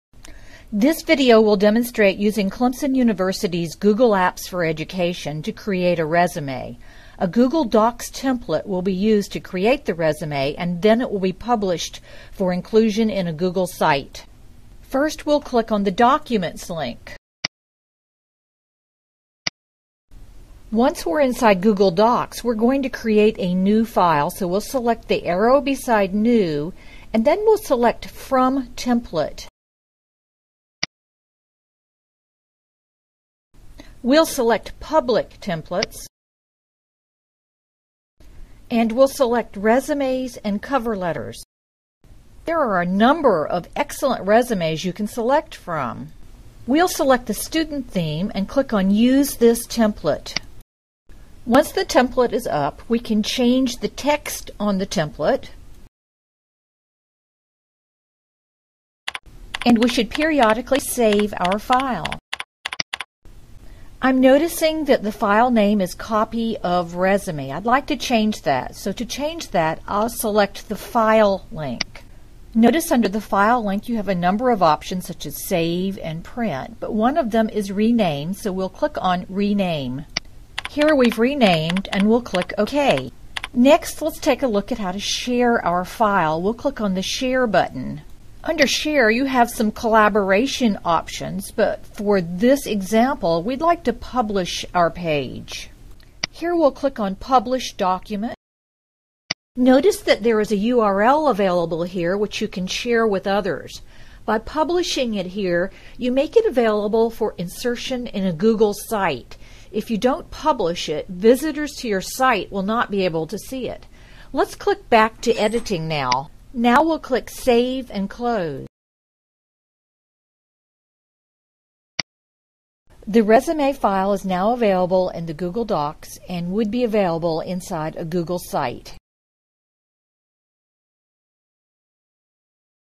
Creating and Publishing a Resume in Google Docs
Demonstration of creating a resume using Google Templates and then publishing it for inclusion in a Google Site.